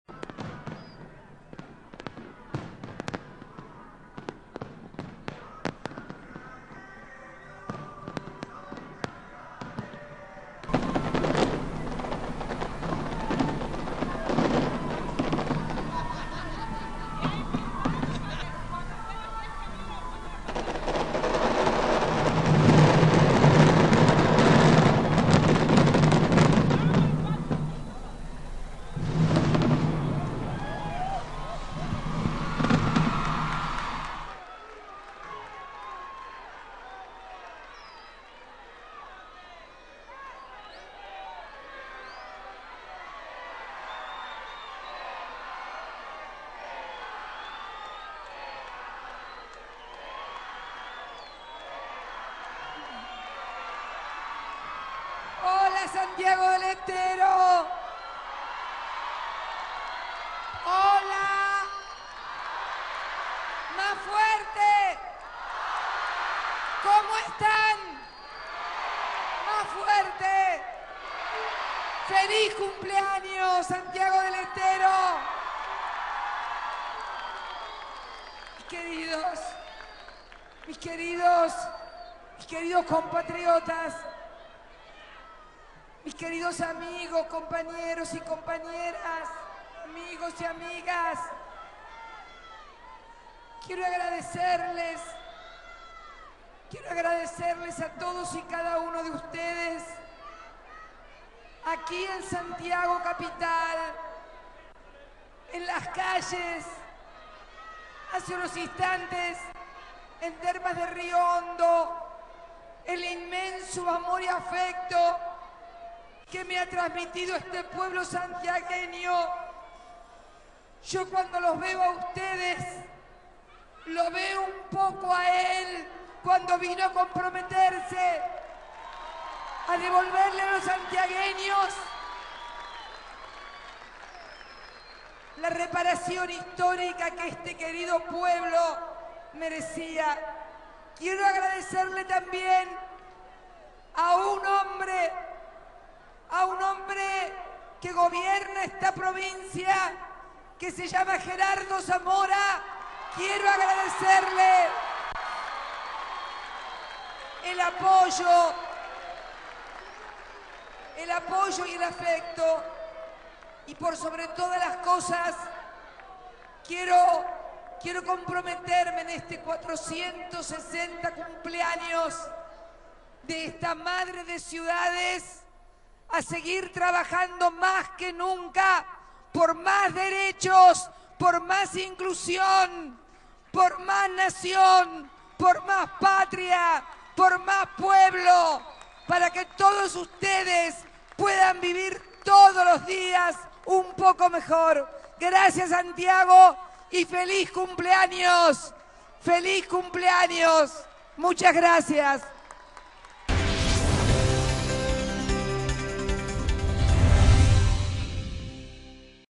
Cristina arribó a la capital santiagueña para presidir los festejos
por 460 Aniversario de la "Madre de Ciudades"

La presidenta Cristina Fernández de Kirchner participó de los festejos por el 460 aniversario de Santiago del Estero, la denominada "Madre de Ciudades", acompañada por funcionarios nacionales y provinciales.

La mandataria recordó al ex presidente Néstor Kirchner y cuando le devolvió a los "santiagueños la recuperación histórica".

Desde el palco de la Casa Nacional del Bicentenario, Cristina Fernández se comprometió ante miles de personas a "seguir trabajando más que nunca por más derechos, por más nación, por más pueblo para que puedan vivir todos los días un poco mejor".